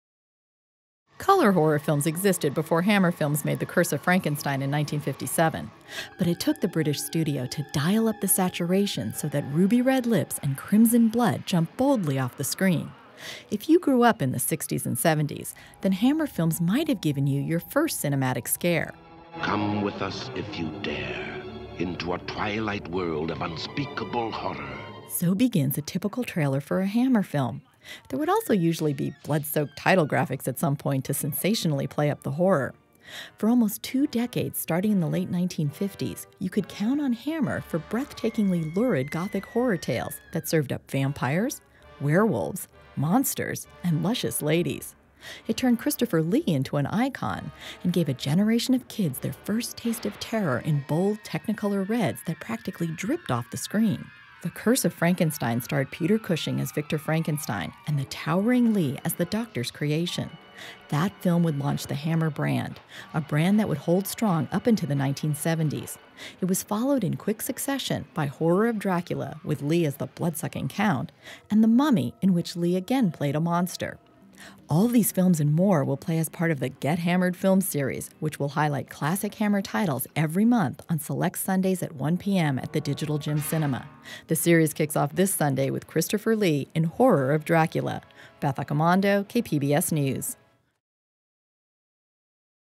Get Hammered Serves Up Tribute To Hammer Films
KPBS film Critic Beth Accomando co-curated a yearlong monthly film series called Get Hammered, which pays tribute to the babes and blood of Hammer Horror. here's a preview.